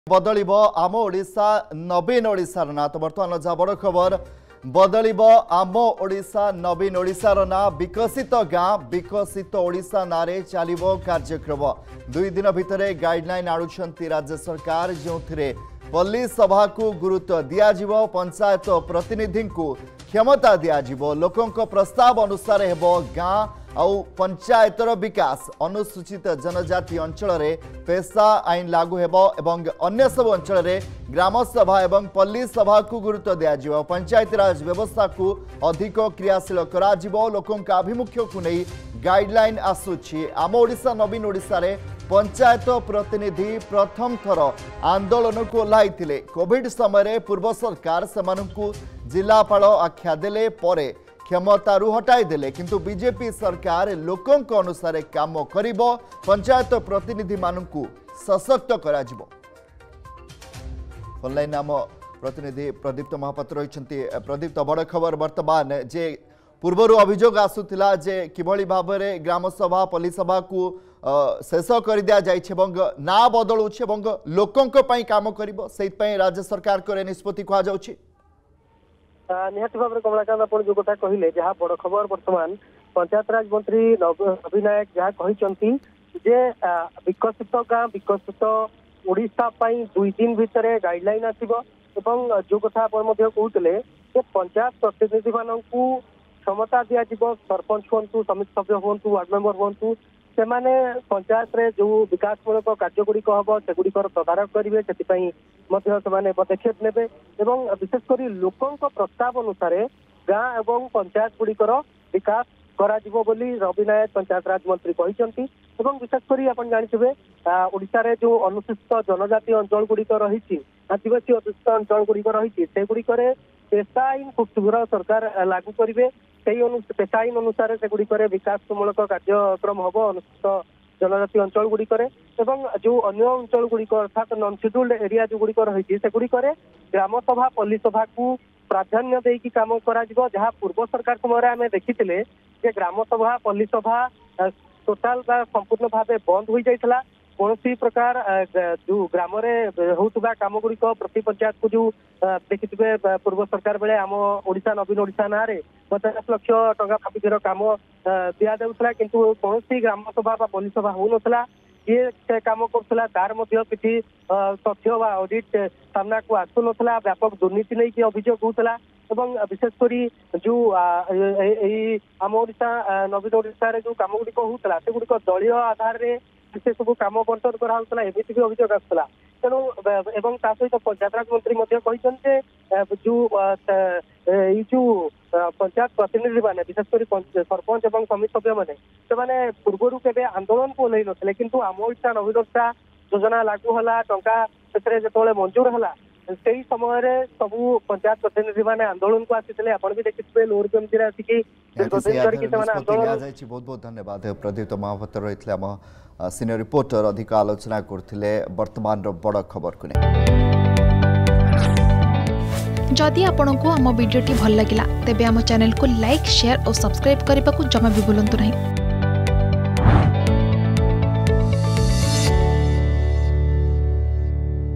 ବଦଳିବ ଆମ ଓଡିଶା ନବୀନ ଓଡିଶାର ନାମ | Ama Odisha Nabin Odisha Will Change To Viksit Gaon Viksit Odisha
ବଦଳିବ ଆମ ଓଡିଶା ନବୀନ ଓଡିଶାର ନାମ । ବିକଶିତ ଗାଁ ବିକଶିତ ଓଡିଶା ନାଁରେ ଚାଲିବ କାର୍ଯ୍ୟକ୍ରମ । ଦୁଇ ଦିନ ଭିତରେ ଗାଇଡ଼ ଲାଇନ ଆଣୁଛନ୍ତି ସରକାର ।